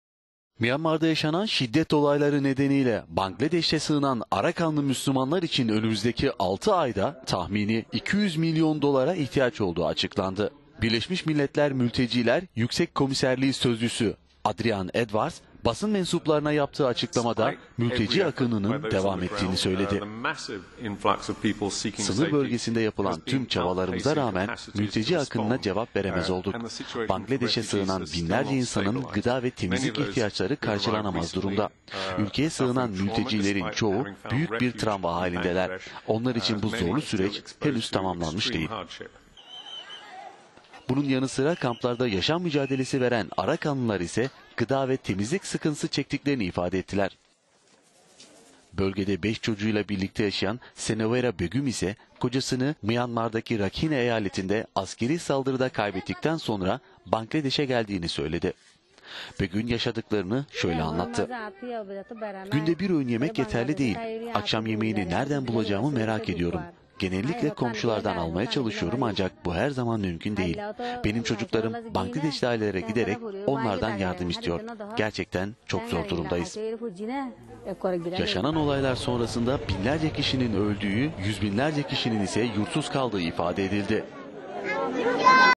BM'den Myanmar'a 200 Milyon Dolarlık destek
Myanmar'da yaşanan şiddet olayları nedeni ile Bangladeş'e sığınan Arakanlı Müslümanlar için önümüzdeki altı ayda tahmini 200 milyon dolara ihtiyaç olduğu açıklandı. 25 Ağustos'tan bu yana Myanmar'dan kaçan ve 400 bini geçen Arakanlı Müslümanlara yardım etmek için Bangladeş ve insani yardım kuruluşlarının verdiği mücadele devam ediyor. Birleşmiş Milletler Mülteciler Yüksek Komiserliği Sözcüsü Adrian Edwards, basın mensuplarına yaptığı açıklamada mülteci akının devam ettiği söyledi.